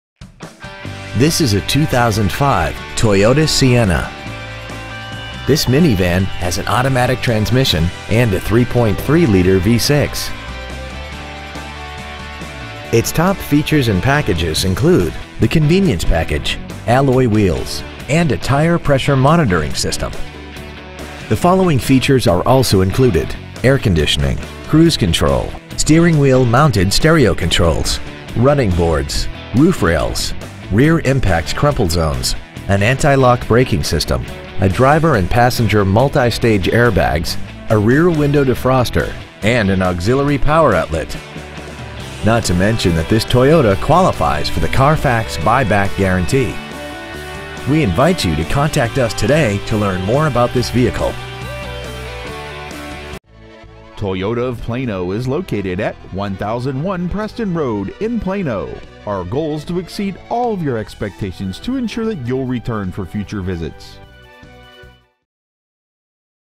2005 Toyota Sienna Plano TX 75093
Year : 2005
 Make : Toyota
 Model : Sienna
 Engine : 3.3L 24-valve MPFI DOHC VVT-i V6 engine
 Trans . : Automatic
 Exterior : Blue
 Miles : 109,372
 Interior : Gray
 Toyota of Plano
 Pre-Owned 2005 Toyota Sienna Plano TX